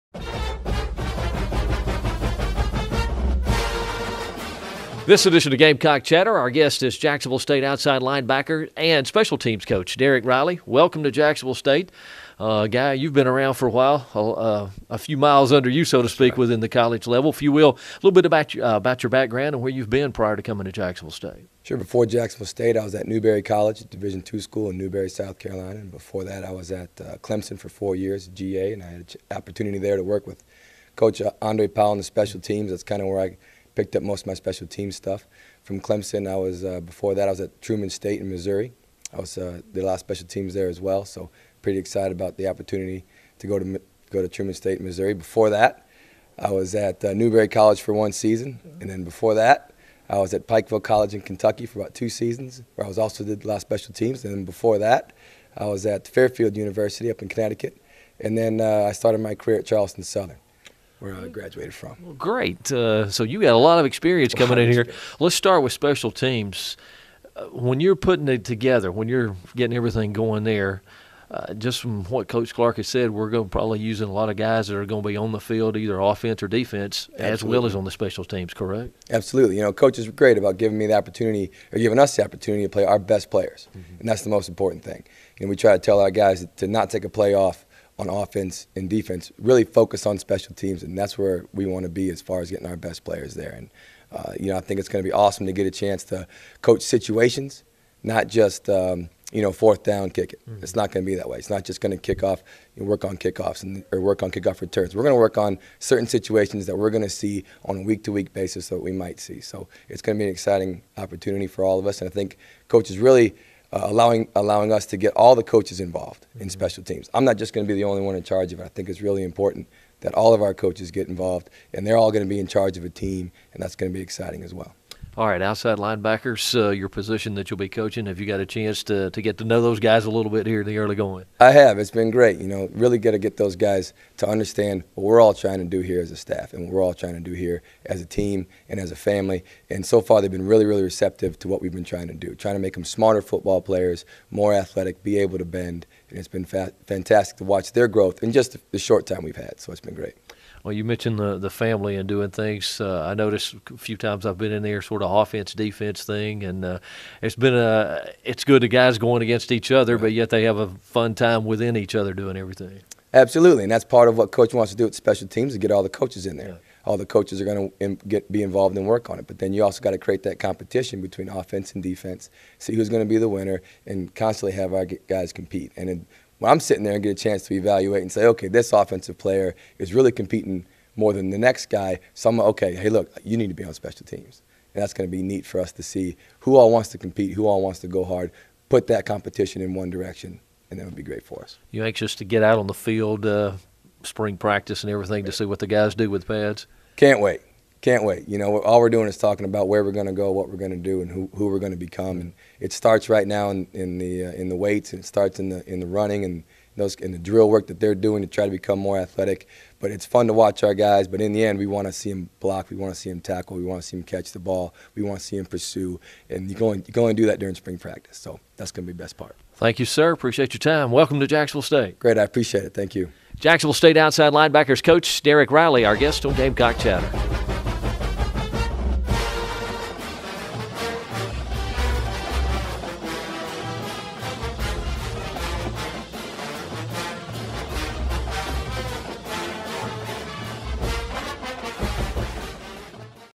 Gamecock Chatter
Mike Parris interviews JSU football outside linebackers/special teams coach Daric Riley.